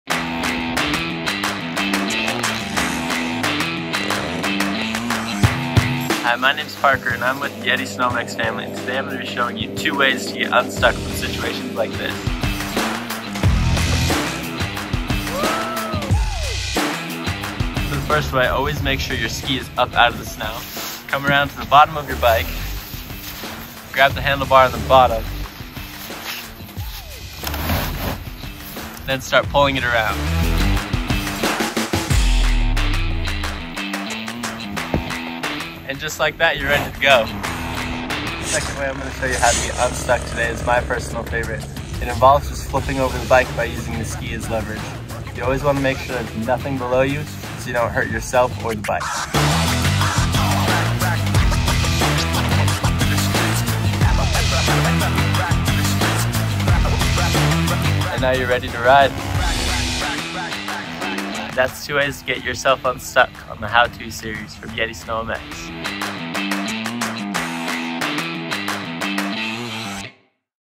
YETI SnowMX - How to get your snowbike unstuck with Parker Earl
Our ambassador Parker Earl explains two of his favourite techniques to get your snowbike unstuck in deep snow areas.

Remember to perform these techniques carefully and to secure the playground to prevent from damaging your snowbike.